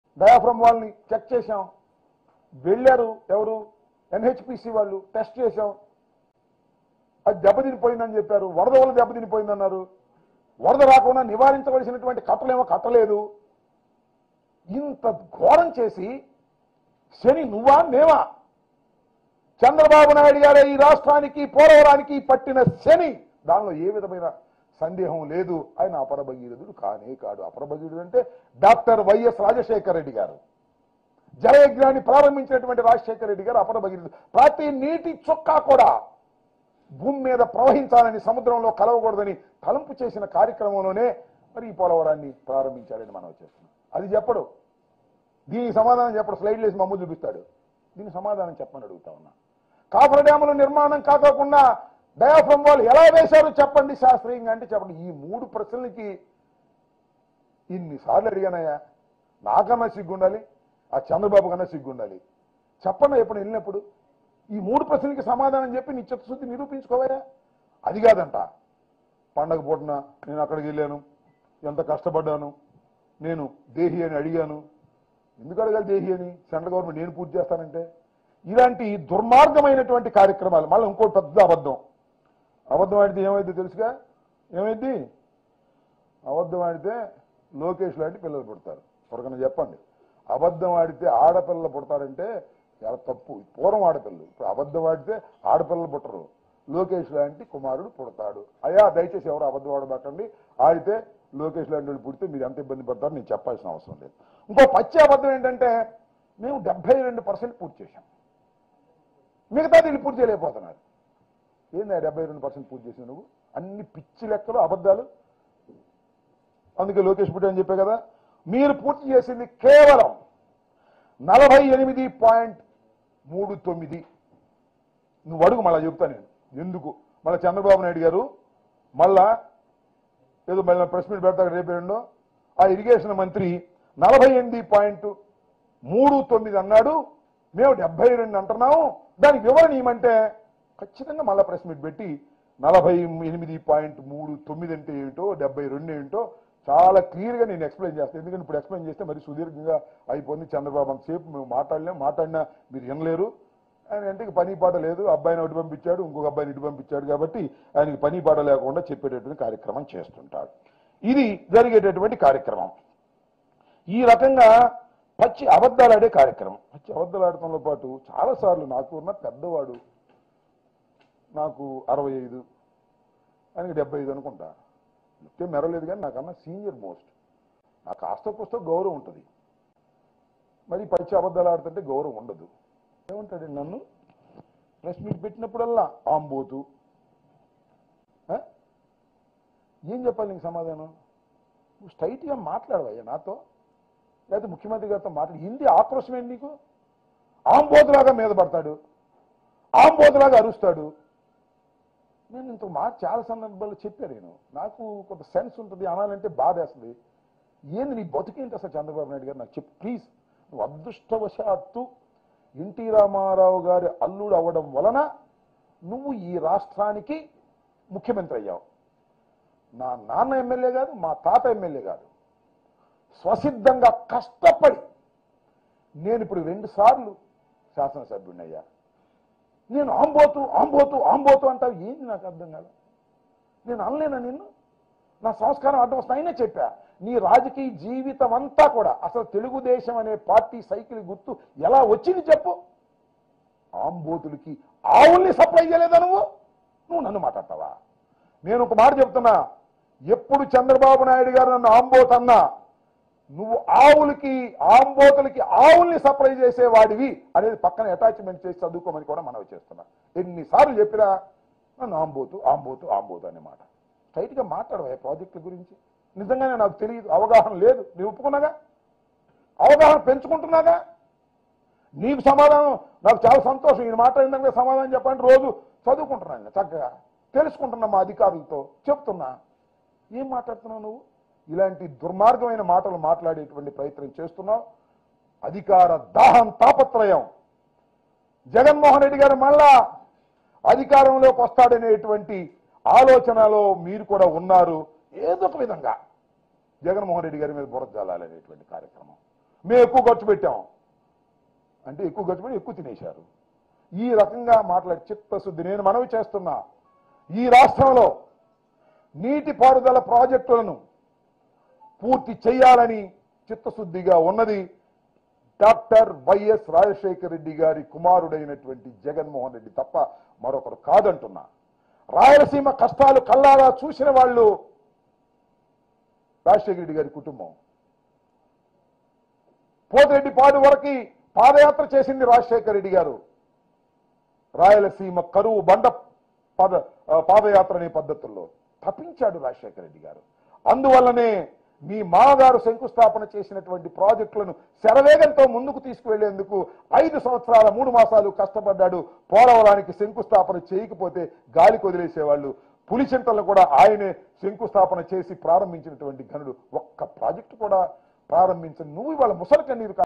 ఎన్టీ రామారావు లేకపోతే నీ బ్రతుకు కుక్క బ్రతుకే | Amabati Rambabu Fire Comments On Chandrababu
LIVE🔴- మంత్రి అంబటి రాంబాబు ప్రెస్ మీట్ | Minister Ambati Rambabu Press Meet | Prime9 News